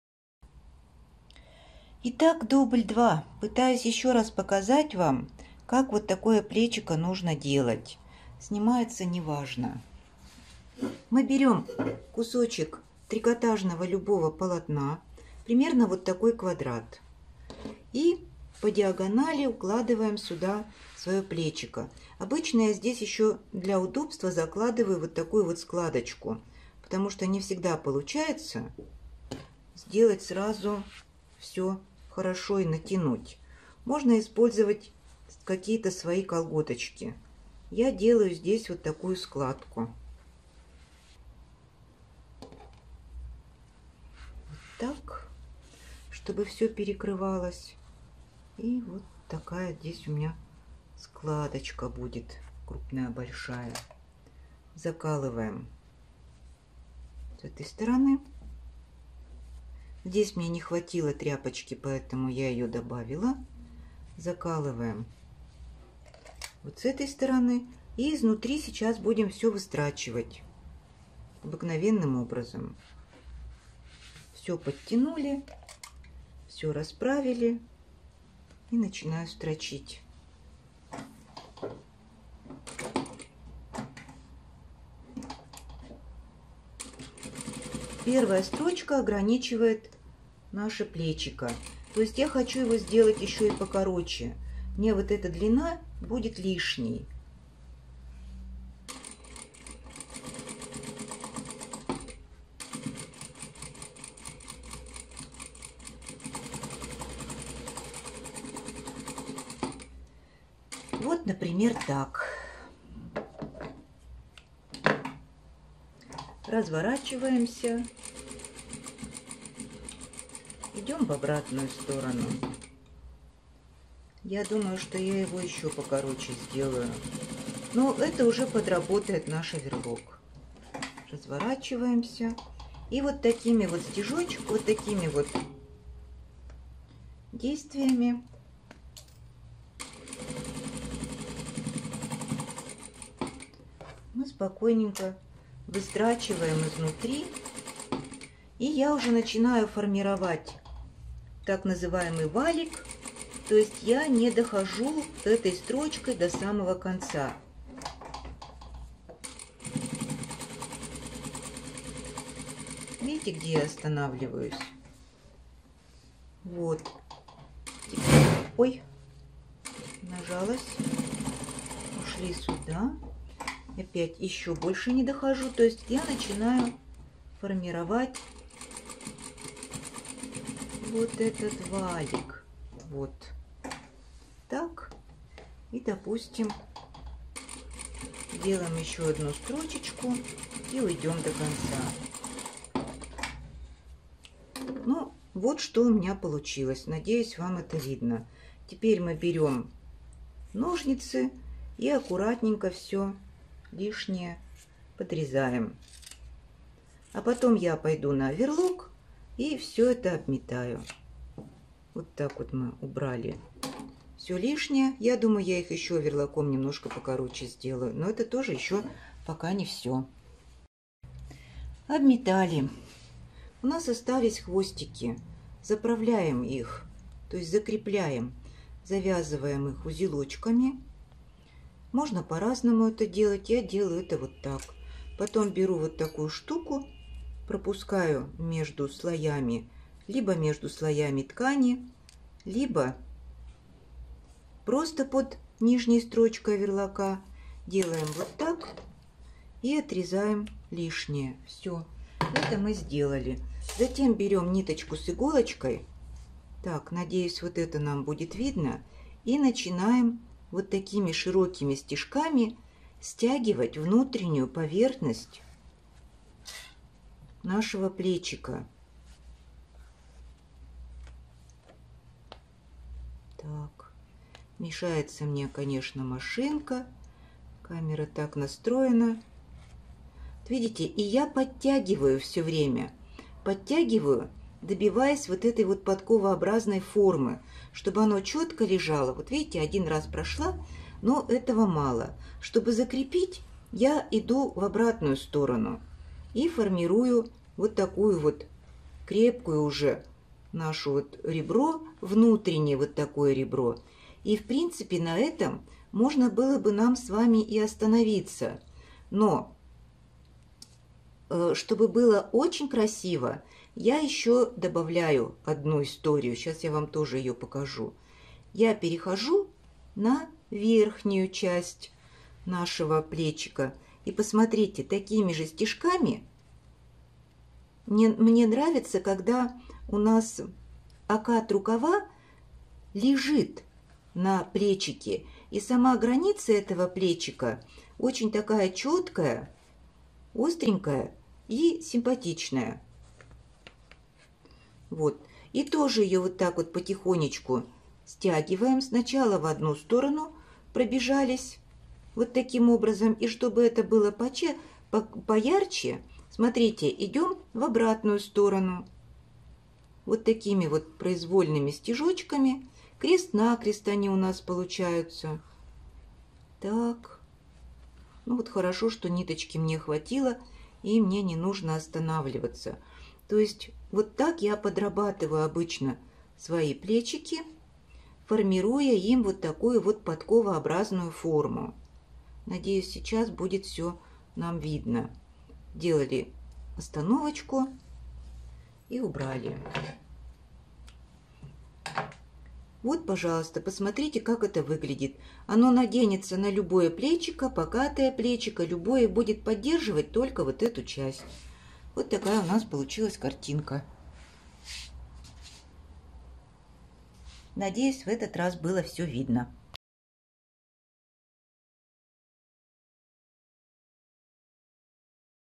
Делаем аккуратные подплечники. Галина Коломейко, Кройка и шитье
Используем покупные подплечники как заготовку.
Вам потребуется кусочек мягкой трикотажной ткани, которым мы обернем подплечники.
Отстрачиваем края на машинке, формируя валик.  Лишнее срезаем.
Окончательную форму придаем вручную.
Совсем немного времени и труда и у вас аккуратный подплечник нужного размера.
Шейте с удовольствием!
Всегда ваша,
Галина Коломейко